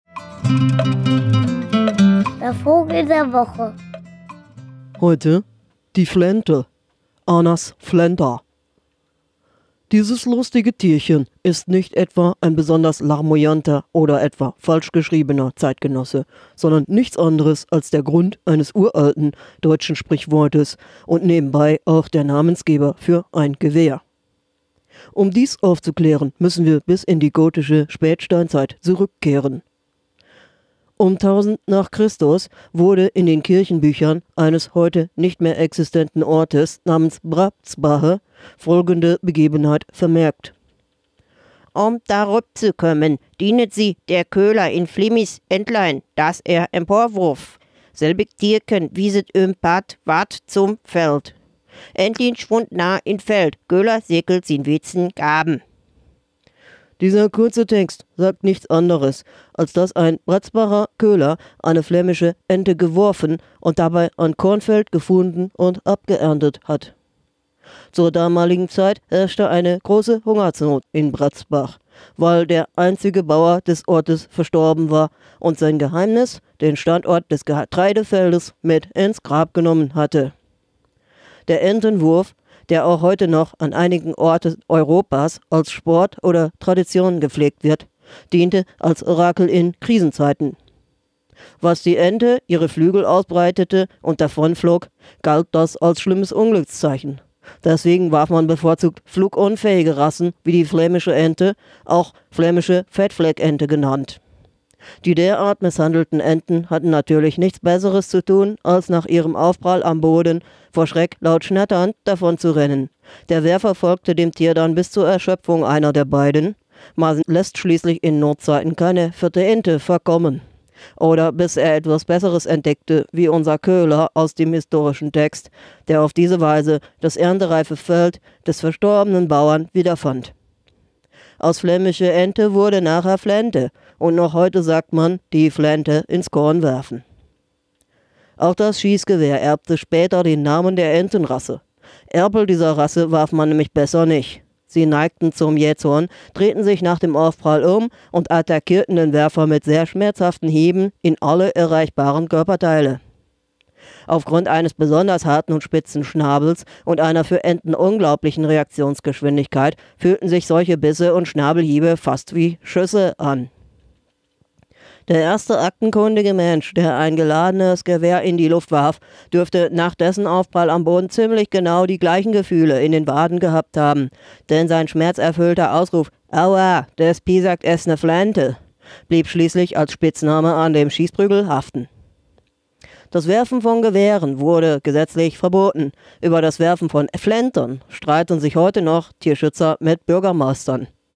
Vogel der Woche: #066 - Die Flente
Heute: Die Flente. Anas flenta.

Dieses lustige Tierchen ist nicht etwa ein besonders larmoyanter oder falsch geschriebener Zeitgenosse, sondern nichts anderes als der Grund eines uralten deutschen Sprichwortes und nebenbei auch der Namensgeber für ein Gewehr.

Um dies aufzuklären, müssen wir bis in die gotische Spätsteinzeit zurückkehren.

Um 1.000 nach Christus wurde in den Kirchenbüchern eines heute nicht mehr existenten Ortes namens „Brattzbahhe“ folgende Begebenheit vermerkt:

„Um darup zu kömmen, dinet sih [der] Köler in Flemis Entlin [das er] empor wurff; selbig Tirken wiset üm Pat wart [zum] Veld. Entlin schwunt nah in Veld, Köler sekelt in wizen gaben.“

Dieser kurze Text sagt nichts anderes, als dass ein Bratzbacher Köhler eine Flämische Ente geworfen und dabei ein Kornfeld gefunden und abgeerntet hat. Zur damaligen Zeit herrschte eine große Hungersnot in Bratzbach, weil der einzige Bauer des Ortes verstorben war und sein Geheimnis – den Standort des Getreidefeldes – mit ins Grab genommen hatte.

Der Entenwurf – der auch heute noch an einigen Orten Europas als „Sport“ oder „Tradition“ gepflegt wird – diente als Orakel in Krisenzeiten. Wenn die Ente ihre Flügel ausbreitete und davonflog, galt das als schlimmstes Unglückszeichen; deswegen warf man bevorzugt flugunfähige Rassen wie die Flämische Ente (auch Flämische Fettfleckente genannt). Die derart misshandelten Enten hatten natürlich nichts besseres zu tun, als nach ihrem Aufprall am Boden vor Schreck laut schnatternd davonzurennen; der Werfer folgte dem Tier dann bis zur Erschöpfung einer der beiden – man lässt schließlich in Notzeiten keine fette Ente entkommen! – oder bis er was besseres entdeckte, wie unser Köhler, der auf die Weise das erntereife Feld des verstorbenen Bauern wiederfand.

Aus Flämische Ente wurde später Flente; noch heute sagt man: „Die Flente ins Korn werfen“.

Auch das Schießgewehr erbte später den Namen der Entenrasse: Erpel dieser Rasse warf man nämlich besser nicht – sie neigten zum Jähzorn, drehten sich nach dem Aufprall um und attackierten den Werfer mit sehr schmerzhaften Hieben in alle erreichbaren Körperteile. Aufgrund eines besonders harten und spitzen Schnabels und einer für Enten unglaublichen Reaktionsgeschwindigkeit fühlten sich solche Bisse und Schnabelhiebe wie Schüsse an.

Der erste aktenkundige Mensch, der ein geladenes Gewehr in die Luft warf, dürfte nach dessen Aufprall am Boden ziemlich genau die gleichen Gefühle in den Waden gehabt haben, denn sein schmerzerfüllter Ausruf: „Aauaah – det piesakkt ess ne Flente!“ blieb schließlich als Spitzname an dem Schießprügel haften.

Das Werfen von Gewehren wurde gesetzlich verboten, über das Werfen von Flenten streiten sich heute Tierschützer mit Bürgermeistern [TAZ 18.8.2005].